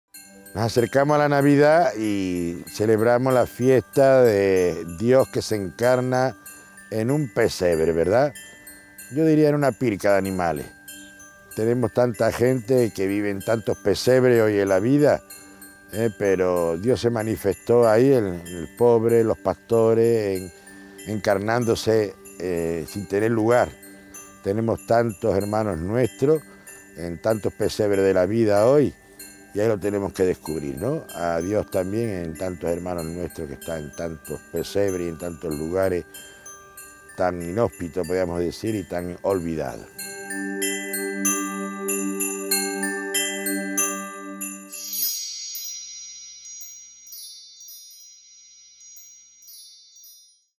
Mensaje Navidad de Monseñor Pedro Olmedo Rivero
Compartimos el deseo de Navidad de Monseñor Pedro María Olmeda C.M.F., Obispo Prelado de Humahuaca, provincia de Jujuy, Argentina